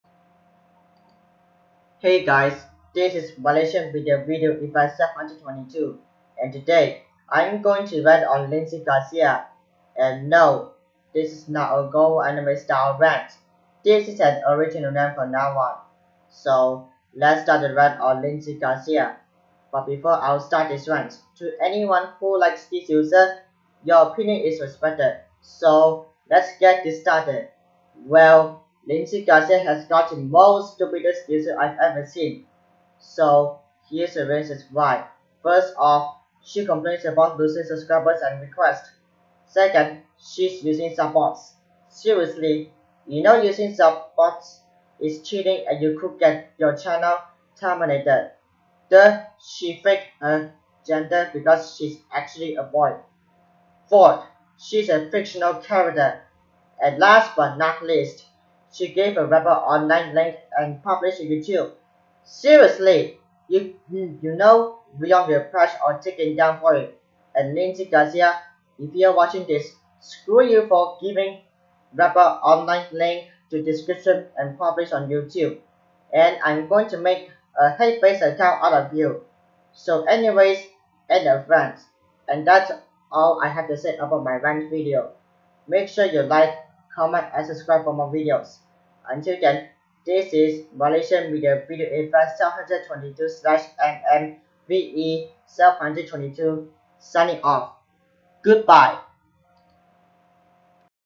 MMVE722 Rants #2: Lindsey Garcia
I use my real voice while I'm ranting on Lindsey Garcia. I will make a hatebase out of her soon a.k.a my emerald clone channel. And Vyond will patch Wrapper Online for what Lindsey Garcia did.

and all the MMVE722 logos